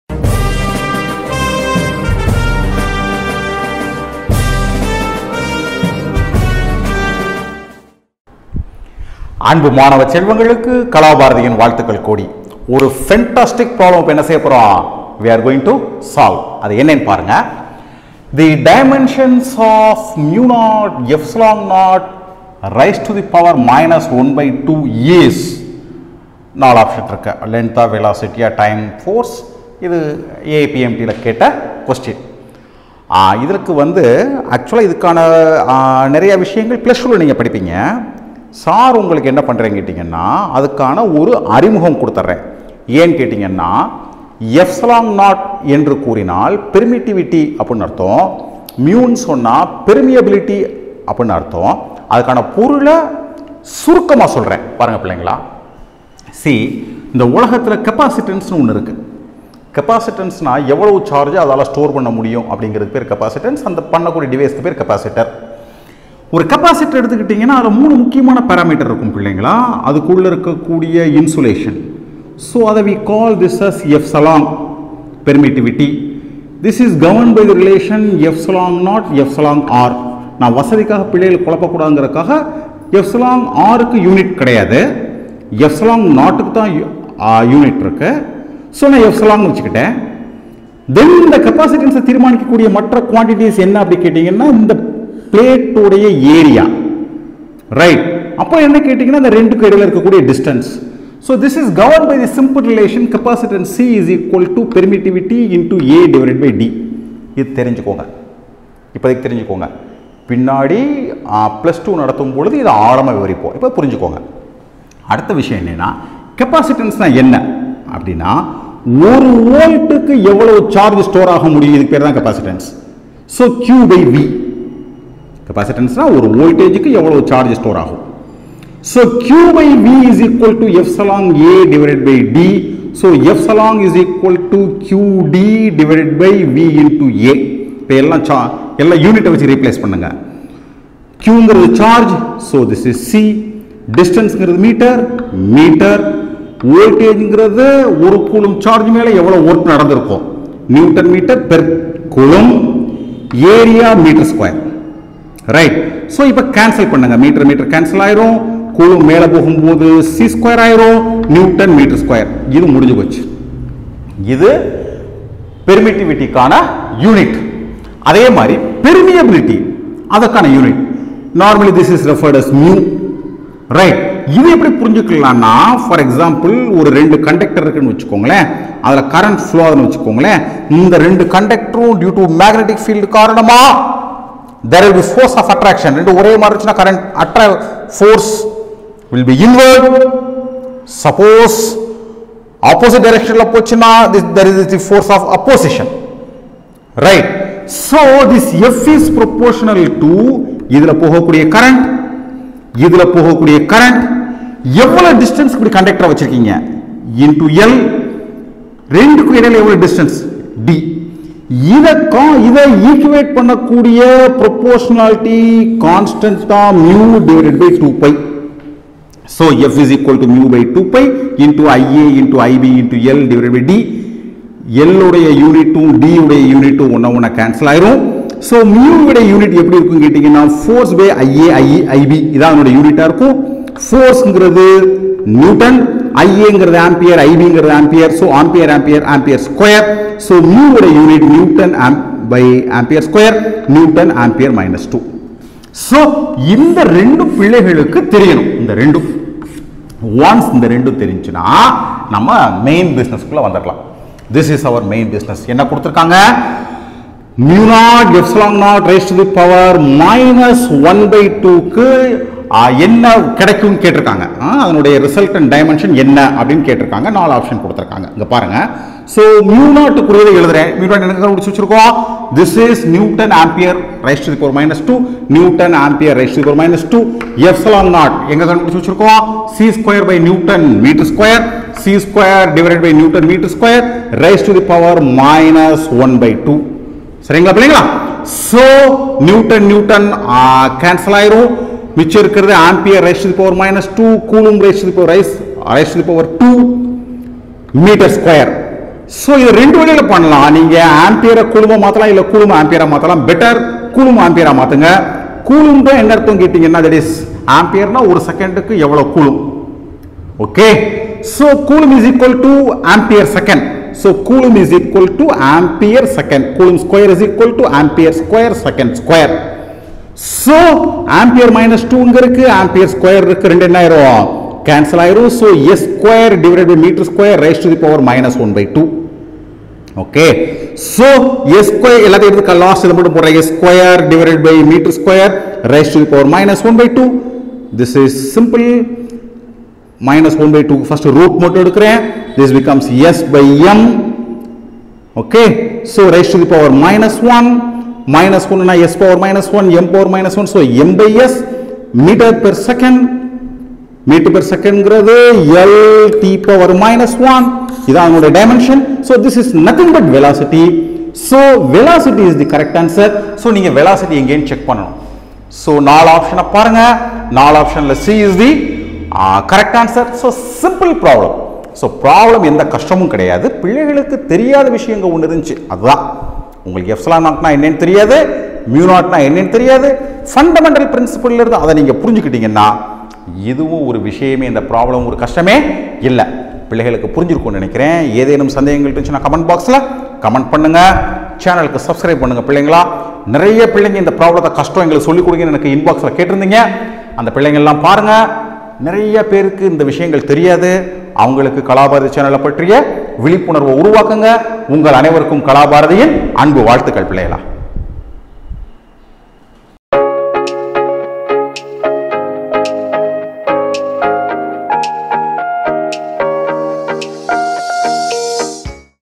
45.Physics | Nature of physical world & measurements | Multiple Choice Question 13
The dimension of (μ0ε0)- 1/2  is                      [Main AIPMT 2011]